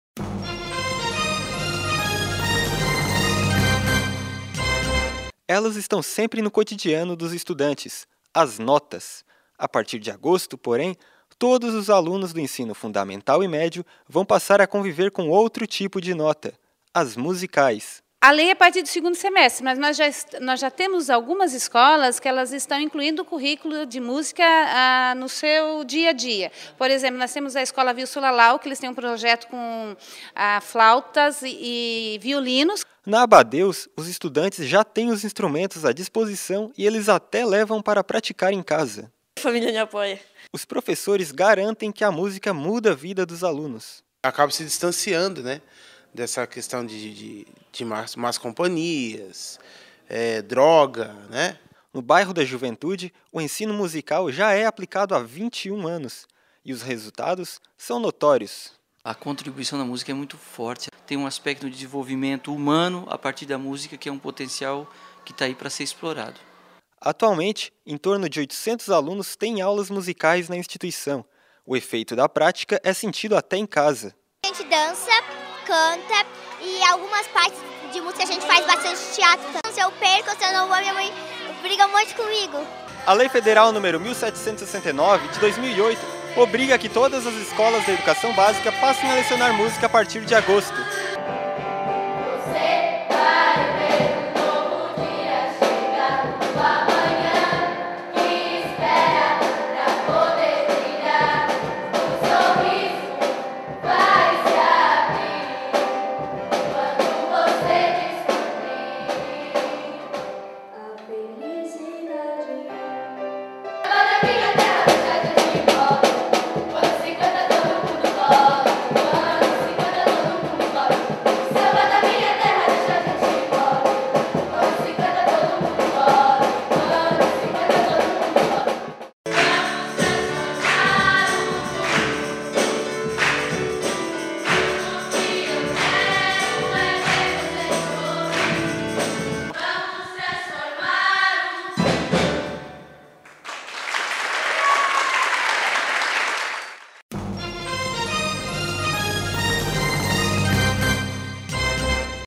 Disciplina de música é implantada nas escolas
Estudantes da rede municipal de Criciúma terão a oportunidade de desenvolver o talento musical.